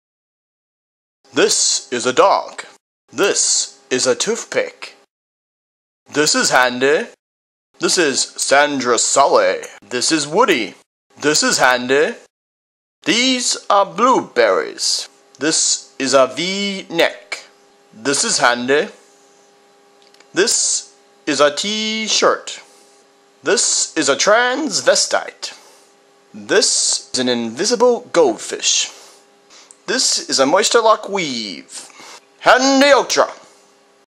Handee Ultra
A send up of the Handee Ultra commerical